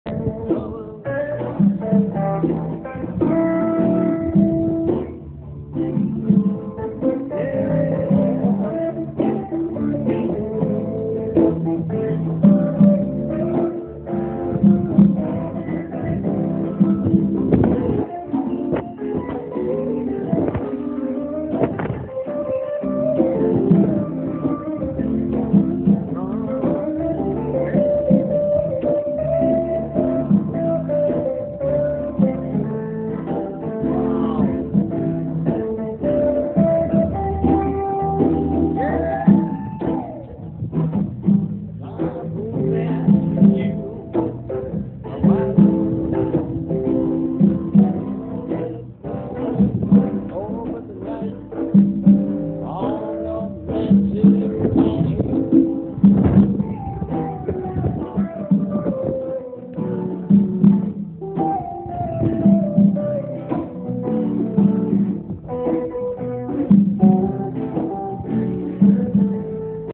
Some light afternoon jazz, Moondance Hawaii . ROLAND HANDSONIC HPD
Moondance on roof waikiki Roland Handsonic percussions